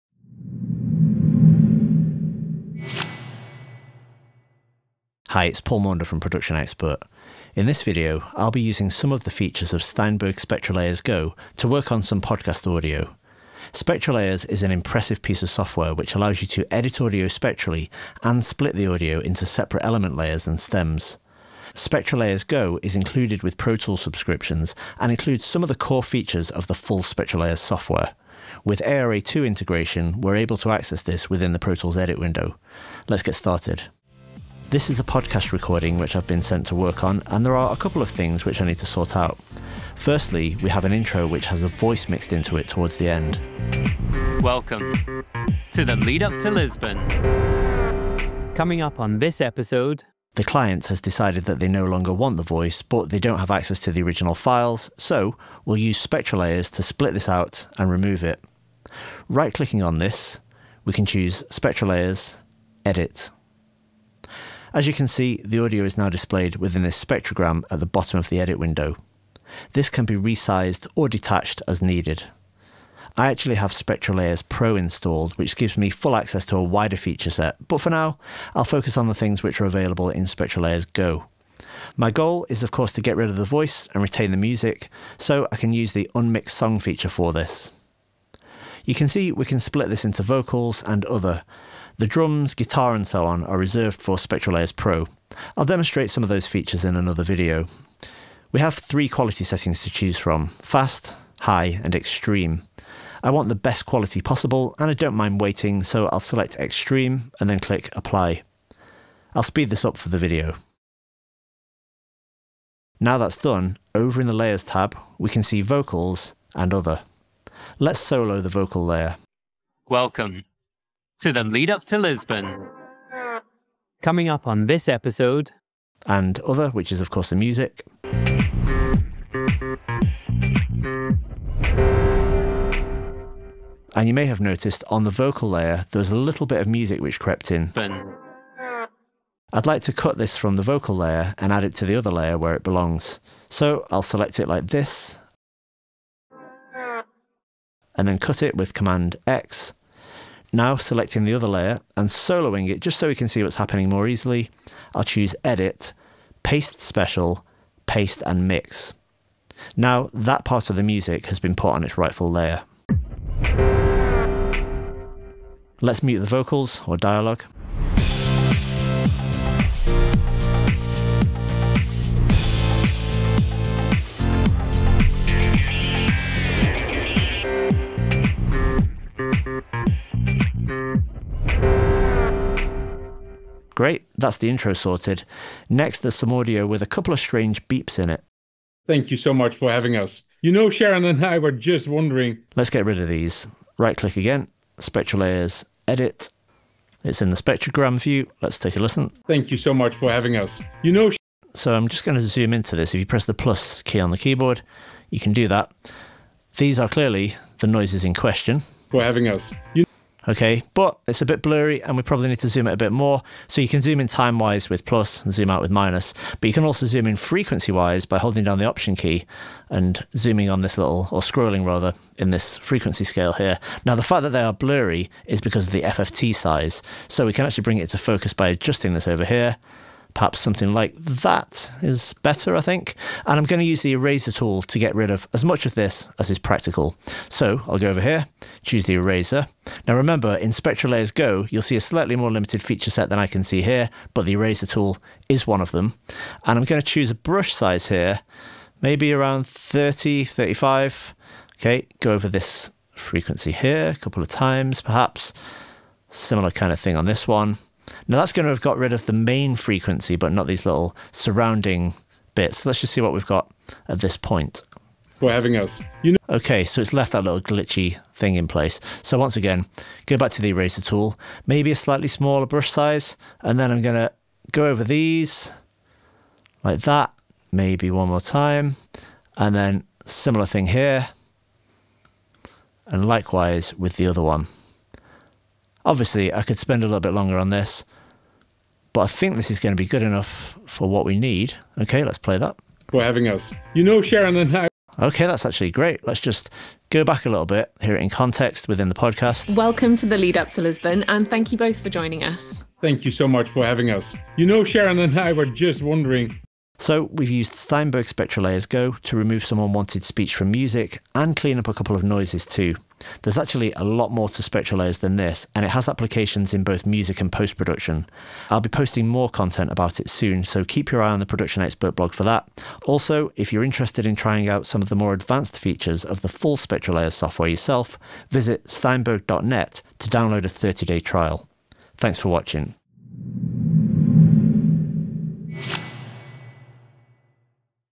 Editing Dialogue Audio With Steinberg SpectraLayers Go
Steinberg SpectraLayers is a spectral audio editor which offers advanced stem separation and powerful layer based editing. A light version of the software, called SpectraLayers Go, is included with Pro Tools.

In this video Paul Maunder demonstrates SpectraLayers Go in action, using the impressive Unmix Song feature to treat some podcast dialogue.

To read the full accompanying article, head to production-expert.com